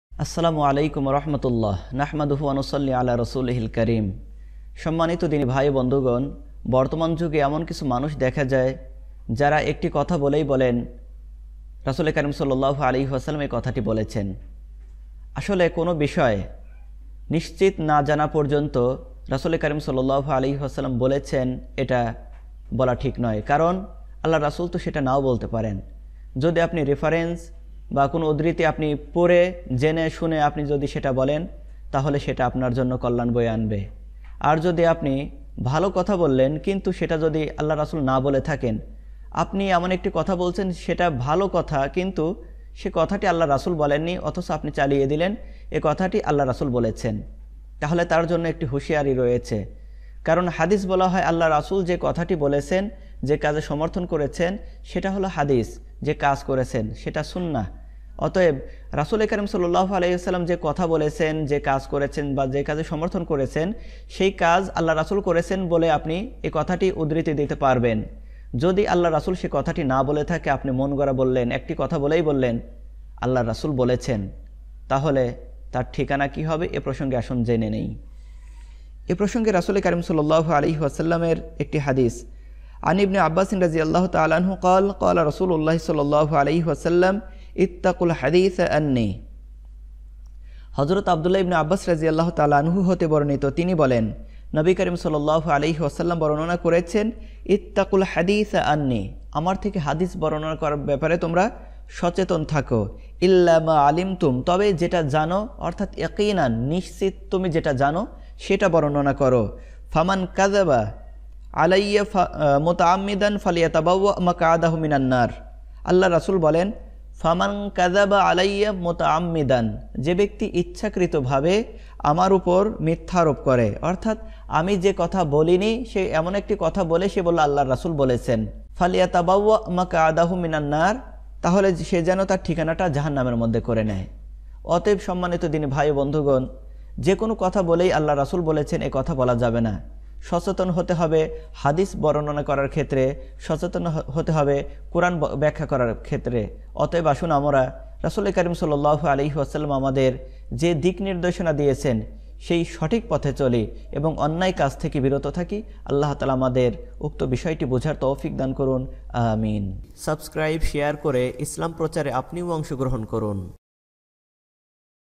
হাদীসের সঠিক ব্যাখ্যা করুন ভুল ব্যাখ্যা করলে জাহান্নাম | Islamic Educational Bangla waz
We are properly Uploaded Very Educational video.
Thank You For being with US.